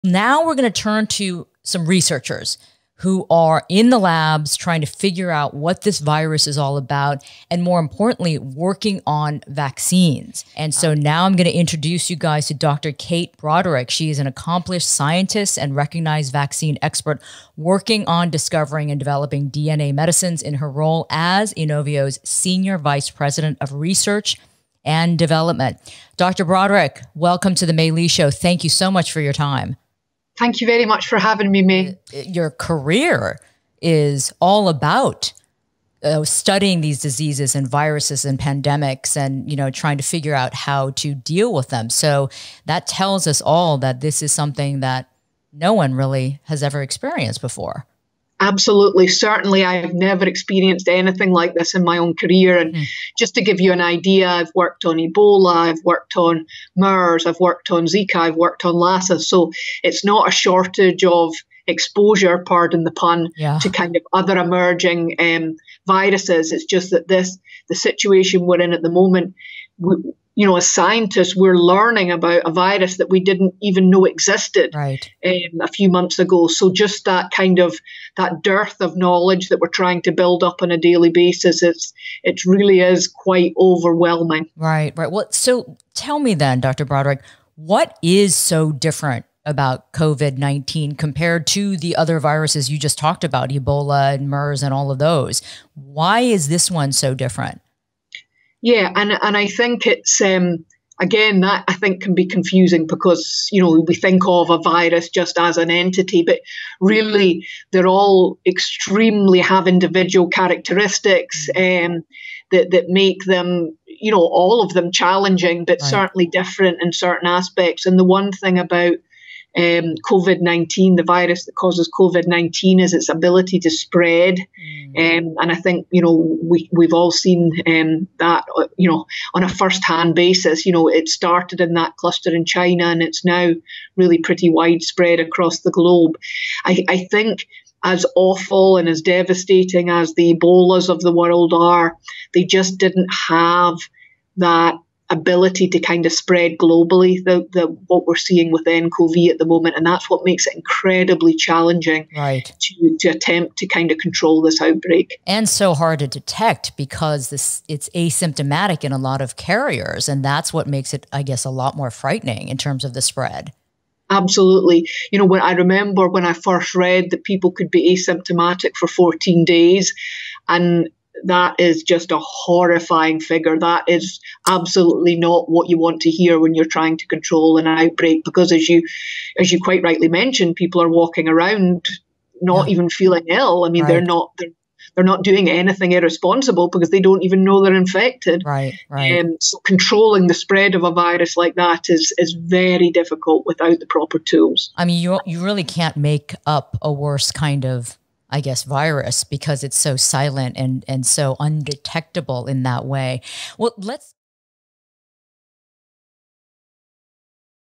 Why COVID-19 is a Bigger Threat Than We May Think
May speaks with Dr. Kate Brodericklead, a leading infectious disease and vaccine researcher who is developing a COVID-19 vaccine. She and her team at Inovio Pharmaceuticals are on the verge of starting clinical trials that, hopefully, will lead to an approved vaccine to defeat this global threat.